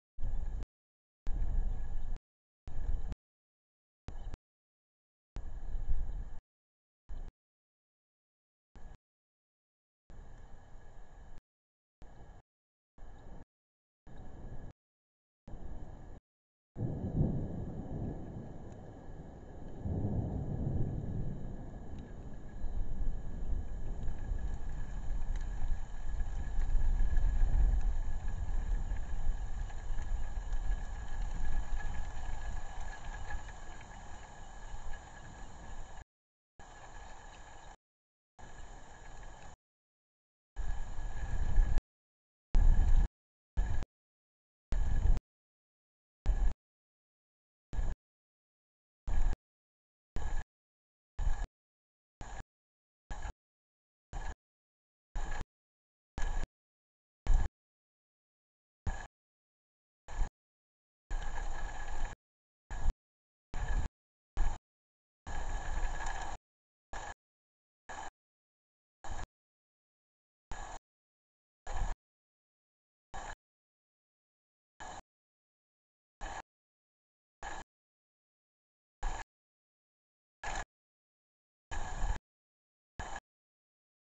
2020 07 14 storm at nest
On July 14, there was a severe storm at the osprey nest. Hail and heavy rain were recorded.
The osprey chicks huddled together to weather the storm.
As seen in the video, our video feed was inconsistent due to the conditions.